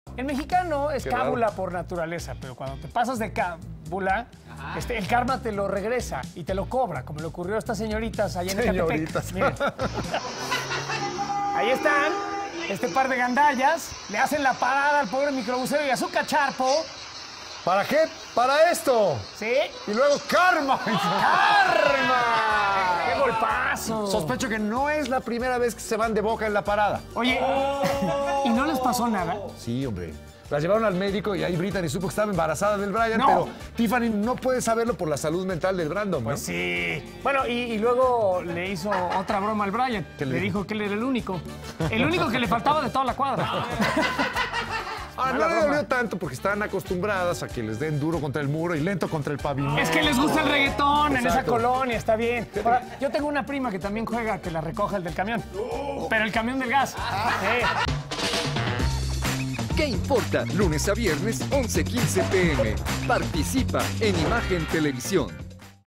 Dos jóvenes se caen tras hacer una broma | Qué Importa
Un par de jóvenes hicieron la parada a un transporte público, pero no para subirse en él, sino para jugarle una broma al conductor. El karma las llevó al pavimento.